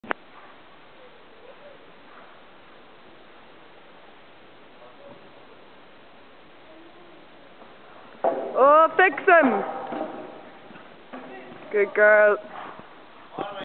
Jumping Blooper
'fix him'...
and i did. lol.
bless charly :) honest horsey. got a bad stride to the first fence so knocked the double but managed to fix him and clear the bigger jump :)